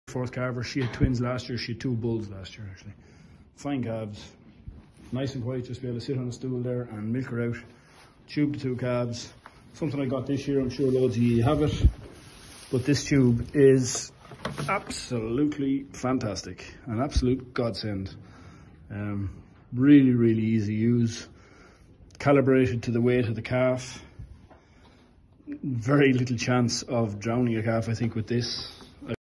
Fleckvieh twins minutes old!
4th calver, twin calves. Heifer and bull, her 2nd set of twins.
Hand milked and calves stomach tubed 3Litres of Colostrum using the Antahi Trusti Tuber stomach tube.